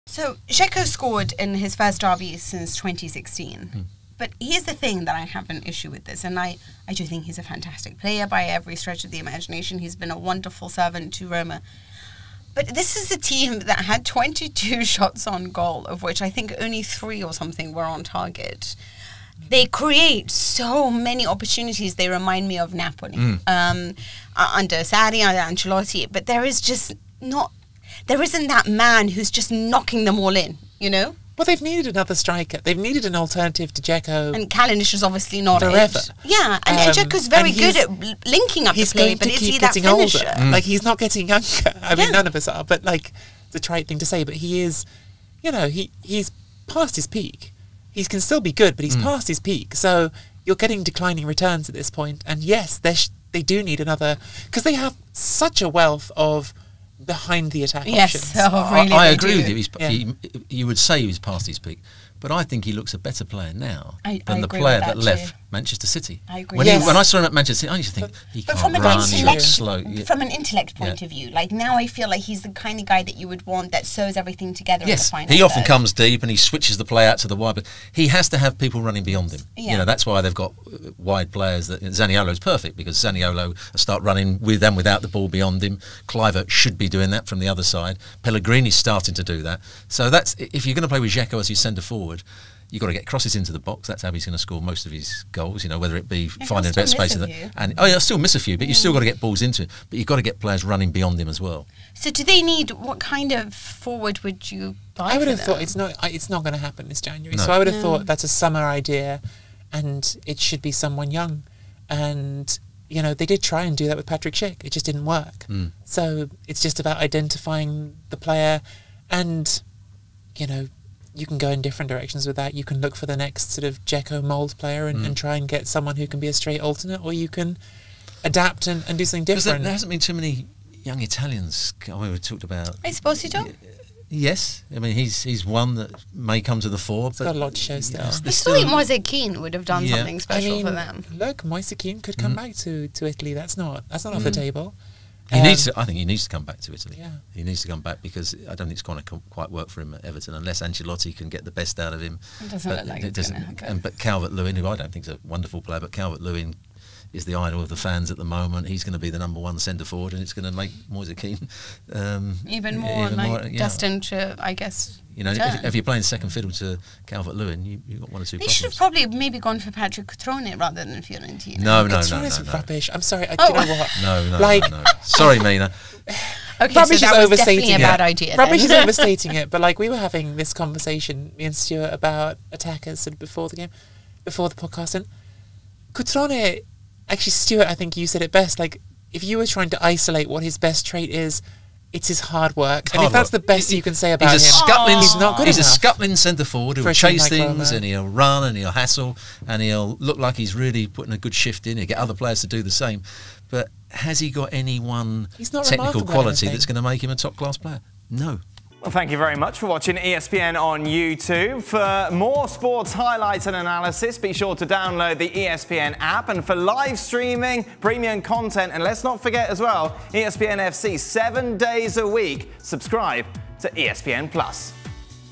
Could Moise Kean help lift Edin Dzeko's scoring burden at Roma? | Serie A
Roma need another striker, so The Serie Awesome Podcast's Mina Rzouki, Nicky Bandini and Stewart Robson assess some potential candidates to help lift the goal-scoring pressure on Edin Dzeko.